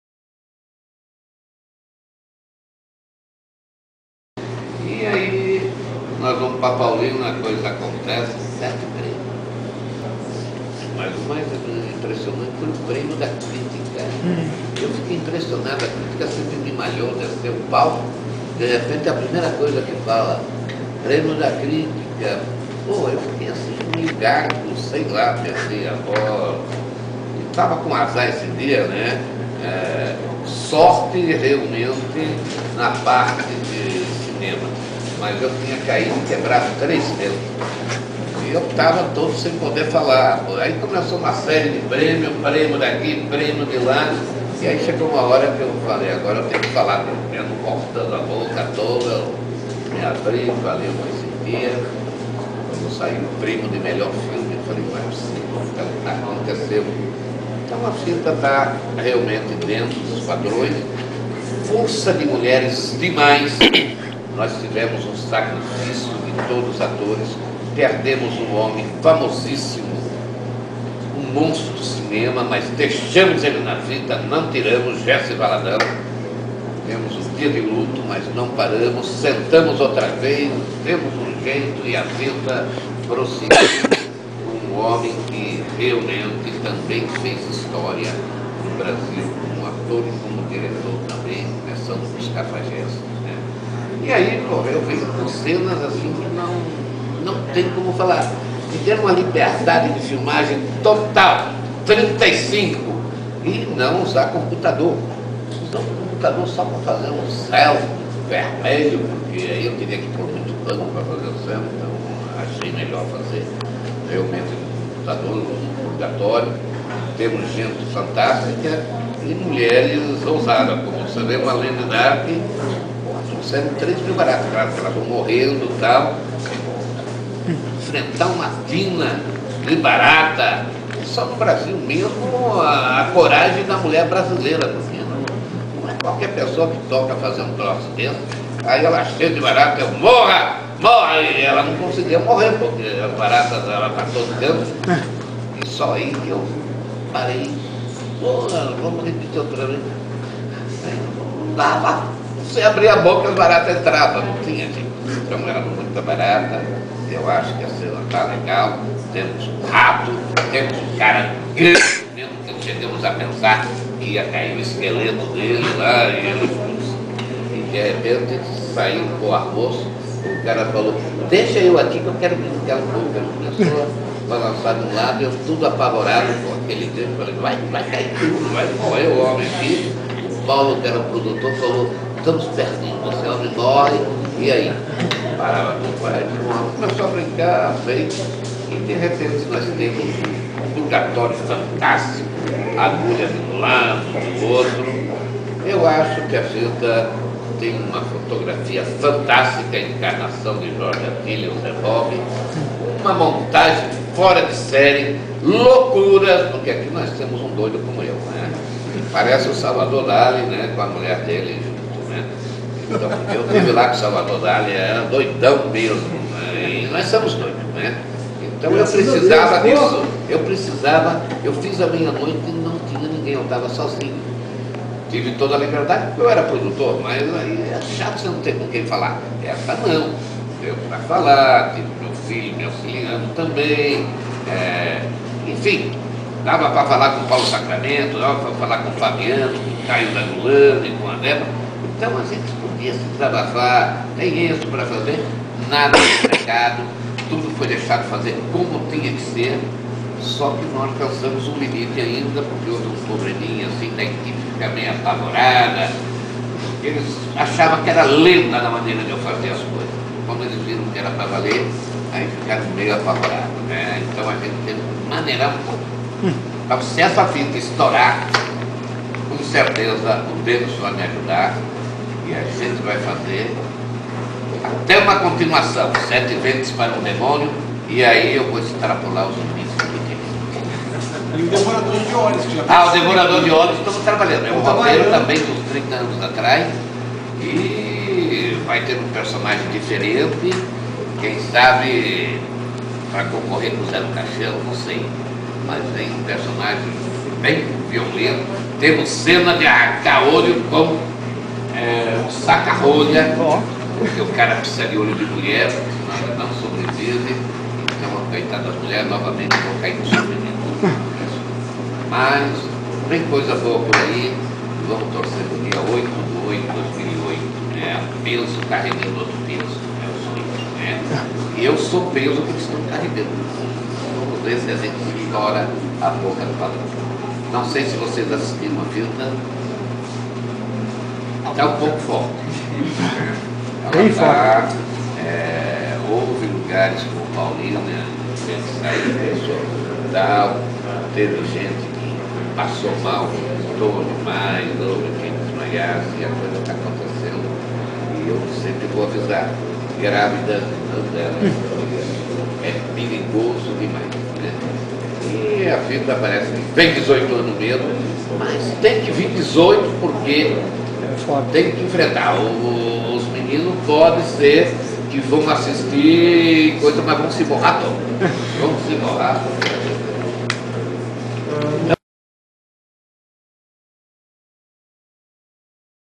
Entrevista com José Mojica Marins - Parte 2
Ainda na coletiva de imprensa realizada durante o Fantaspoa 2008, em Porto Alegre, Felipe M. Guerra (representando o site Boca do Inferno) continua incomodando seu grande ídolo, o cineasta brasileiro José Mojica Marins, vulgo Zé do Caixão.

Aqui, Mojica fala sobre como ficou surpreso com o sucesso que ENCARNAÇÃO DO DEMÔNIO vem fazendo junto à crítica, sobre a extrema violência do filme e também entrega um pouco sobre seus futuros projetos: O DEVORADOR DE OLHOS e SETE VENTRES PARA UM DEMÔNIO!!!

Como plus a mais, legendas que permitem compreender melhor o que Mojica está falando.